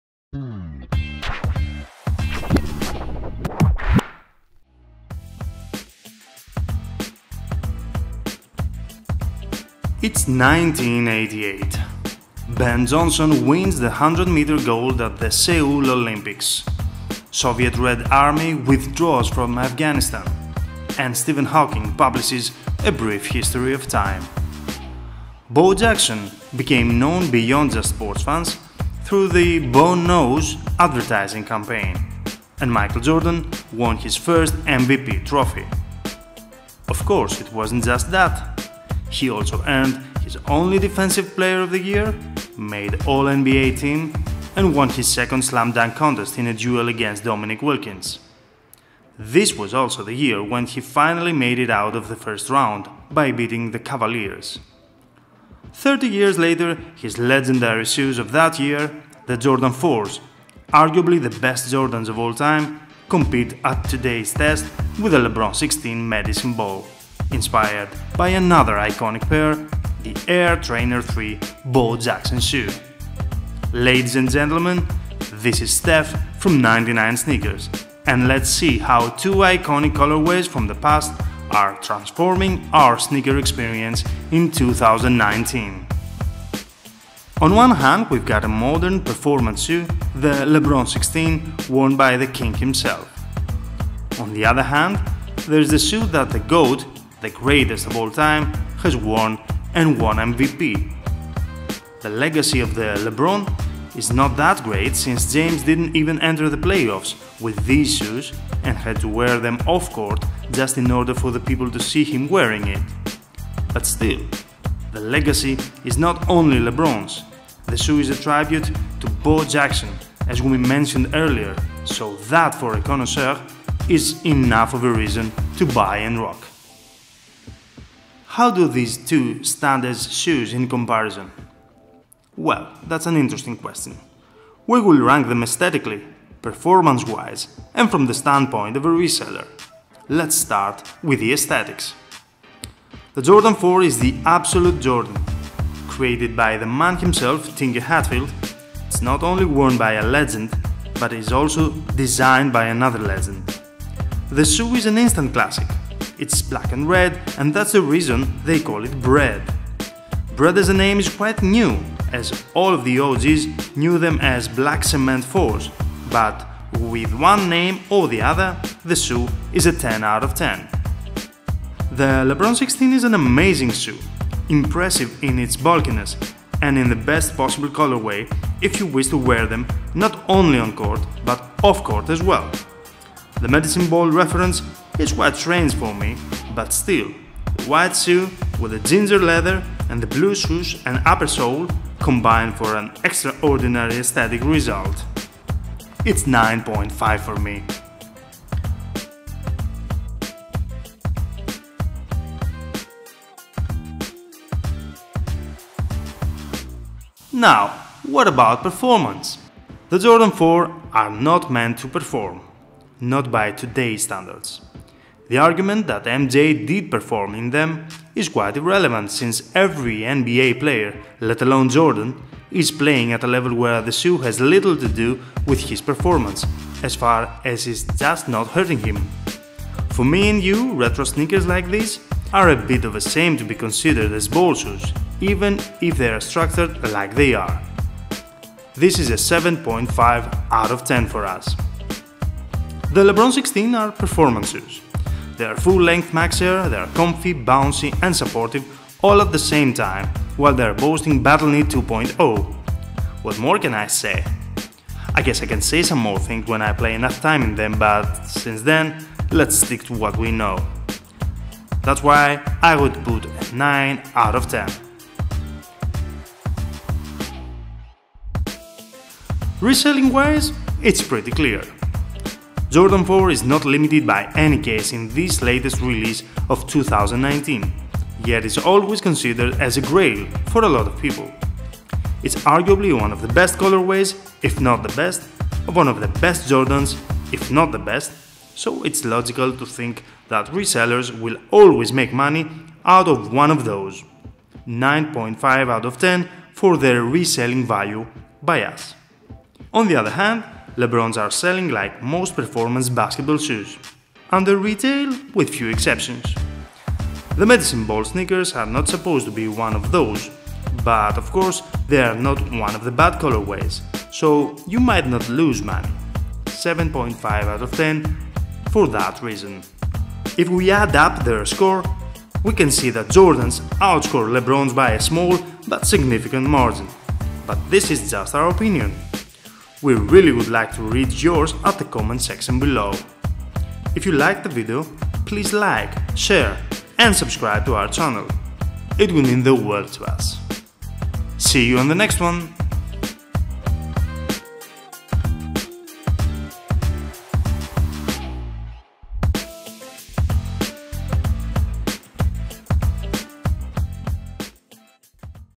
IT'S 1988 AGAIN! BRED vs MEDICINE BALL
On one hand, we've got a modern performance shoe.
On the other hand, GOAT's MVP winning shoe.
Ladies and gents, this is a comparison between the Air Jordan 4 Bred and the Lebron 16 Medicine Ball.